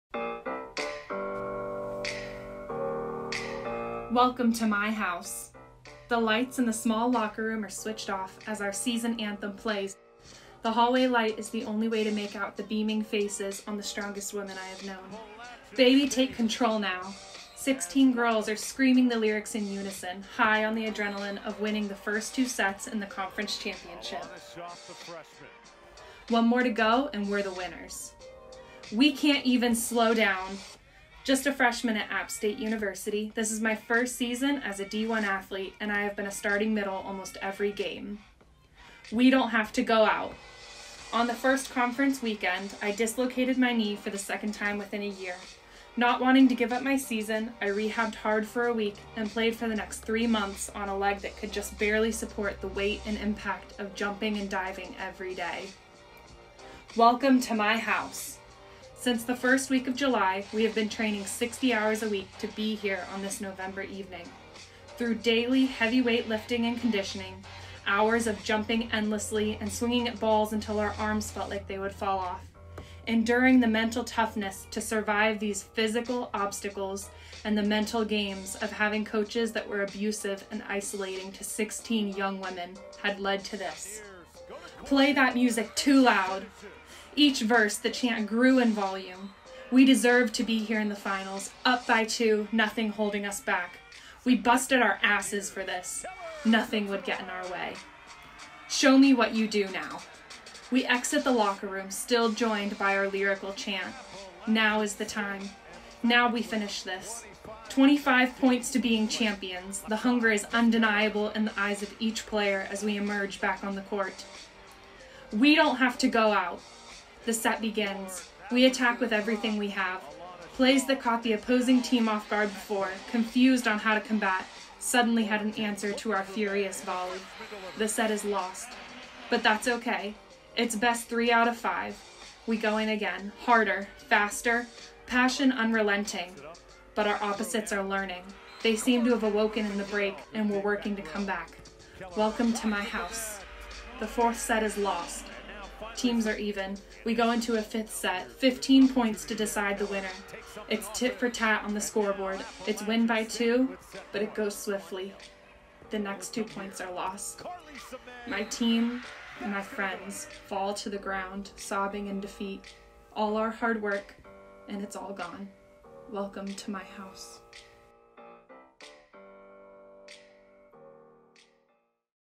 Our House: The Volleyball Conference Championship
This is my midterm personal story project for Digital Storytelling Spring 2020 (MMC6369). My story is about being a freshman middle playing at the Sun Belt conference championship in the finals match between Appalachian State University and Arkansas State University.